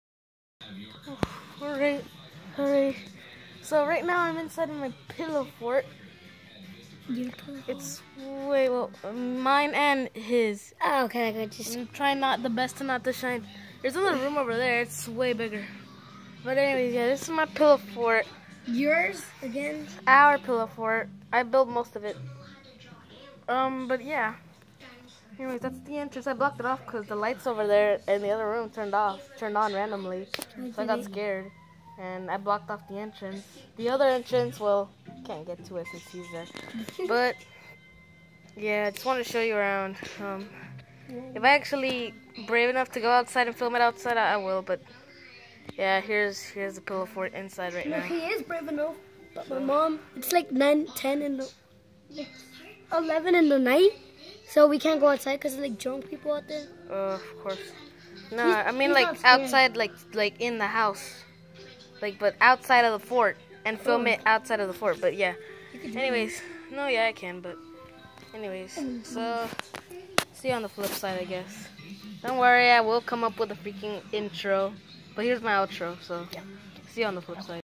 Inside our pillow fort
Having a sleep over at my cousins house a.k.a. another you tuber